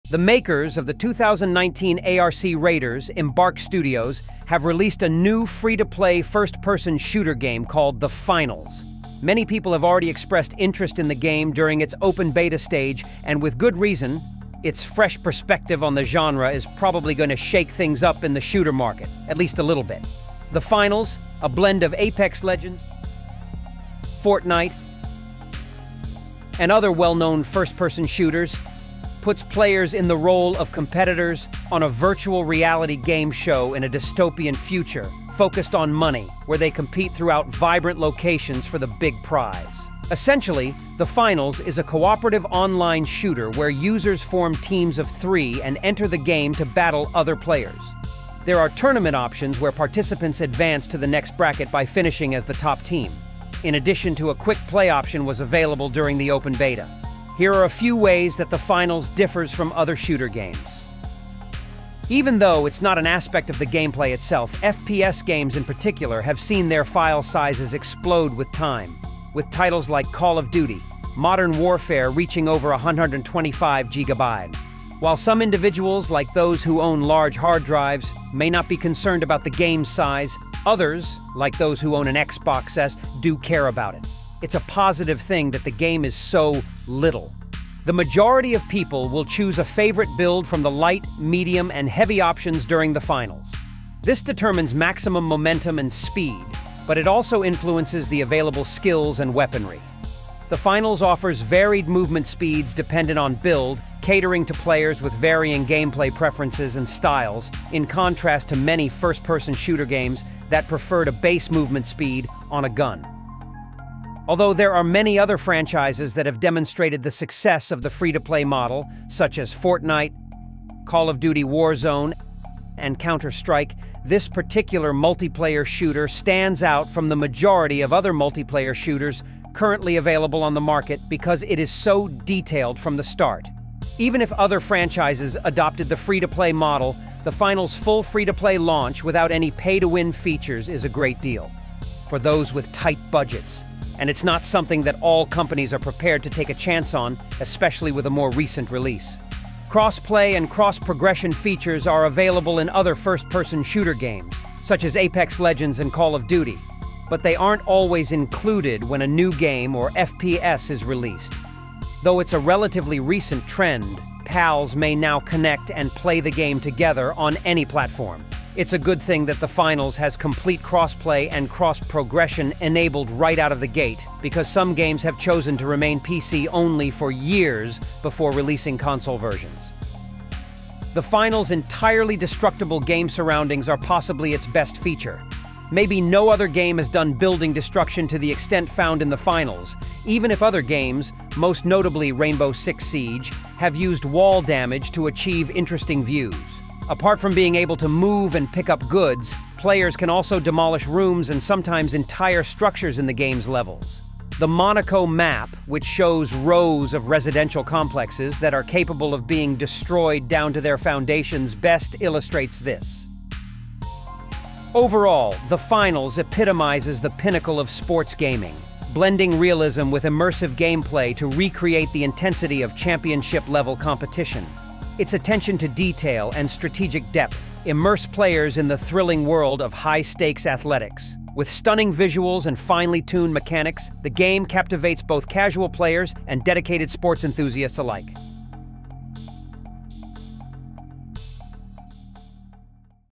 The Finals Review: A Gamer's Perspective!
Welcome to the ultimate guide to 'The Finals'! Join us as we dive into the heart-pounding world of 'The Finals' gameplay, exploring every aspect from the open beta to the full release. In this video, we'll provide an in-depth review of 'The Finals,' covering its beta stages, gameplay mechanics, and the thrilling destruction this game brings to the table.

Whether you're a seasoned player or new to the game, this review aims to offer insights into the game's mechanics, graphics, and overall experience.

Get ready to witness the chaos and action as we uncover what makes 'The Finals' a game-changer in the gaming world.